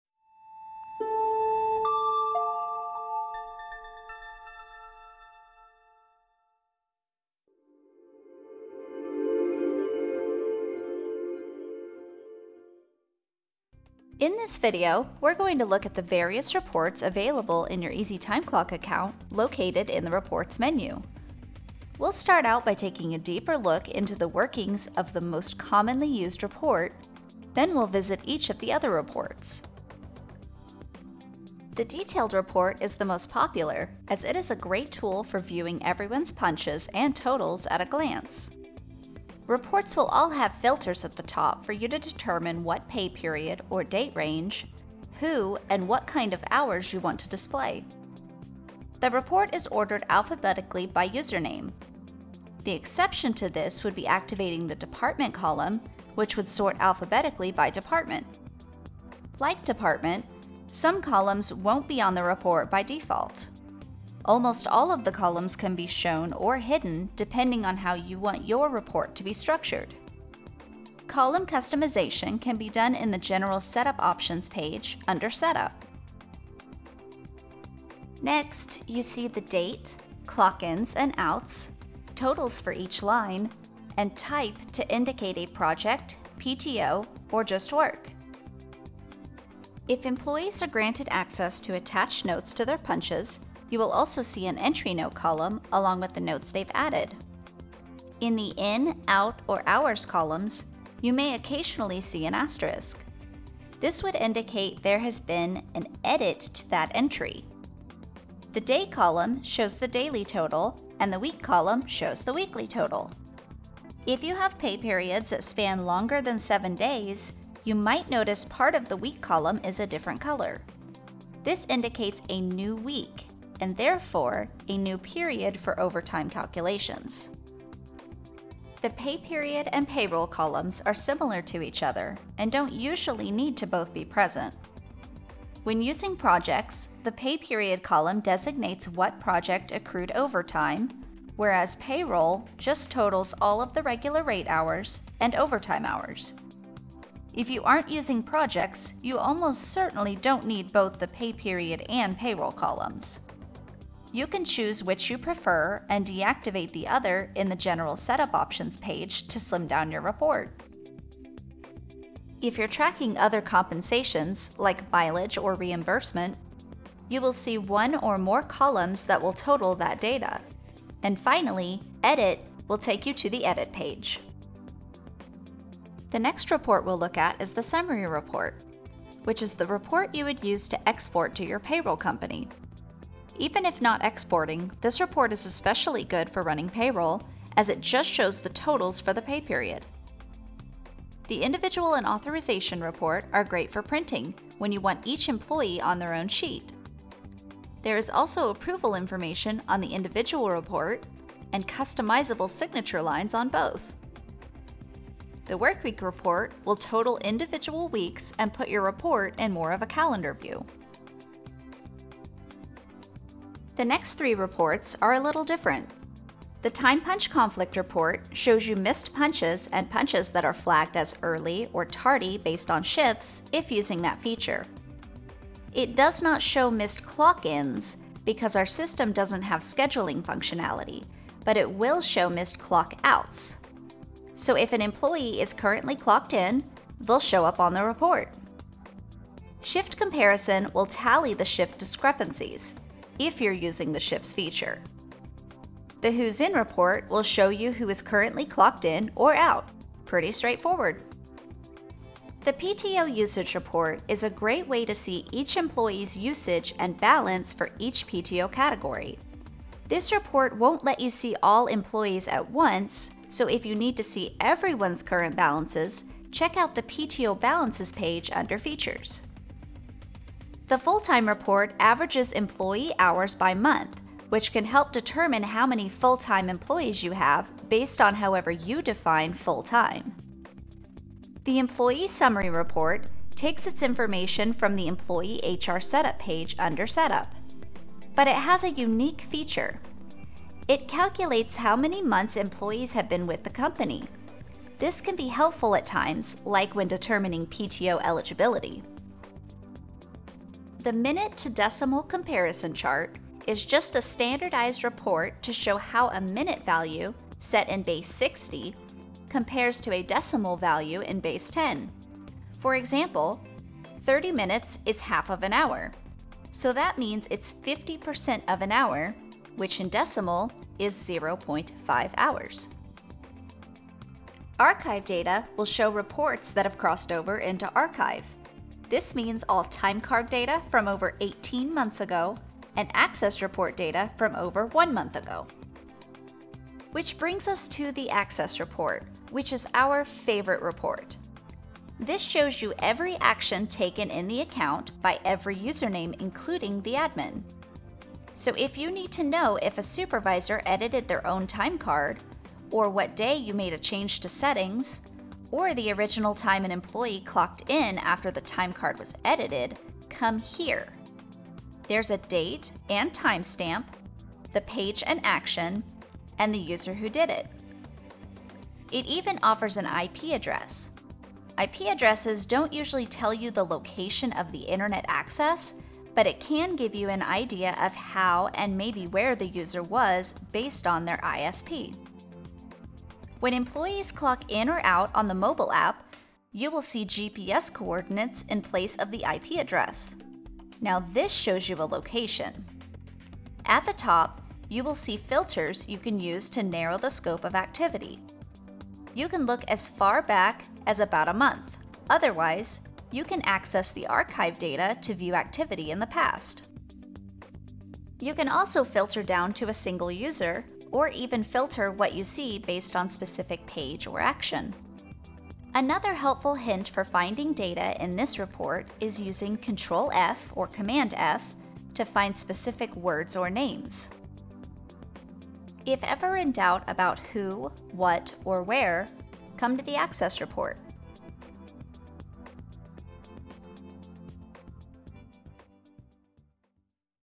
Reports (V-15)
All reports can be found in the Reports menu. In this video, we'll show an overview of what each report contains and what it's used for.
00:00 Intro
00:30 Detailed Report (most popular report)
02:55 Summary Report (to run payroll)
03:10 Other Time Card Reports (print-and-sign approvals)
03:32 Other Reports (Who's In report)
04:12 PTO Usage Report
05:00 Convert time to decimal value
05:23 Archive (time cards older than 18 months)
05:36 Access Report (audit log of account activity)